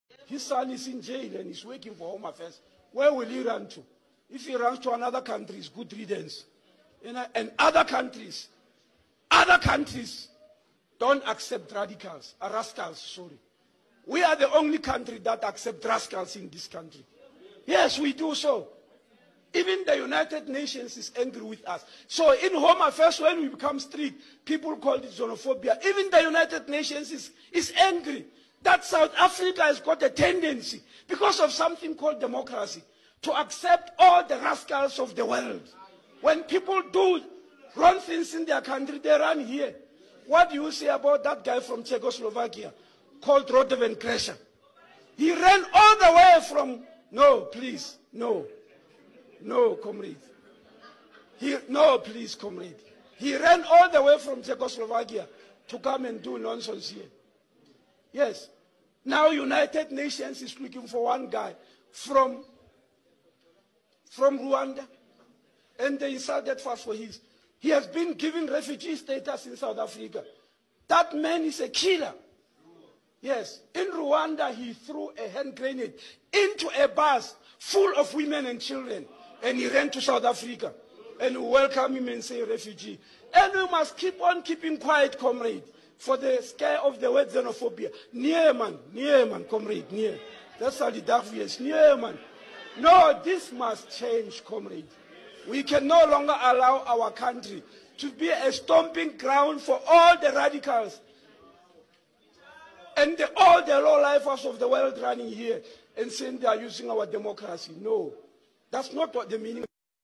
Minister of home affairs tired of illegal Immigrants and the corrupt ANC
Home affairs minister on the cancellation of zimbabwean permits.
New Operation Dudula News Channel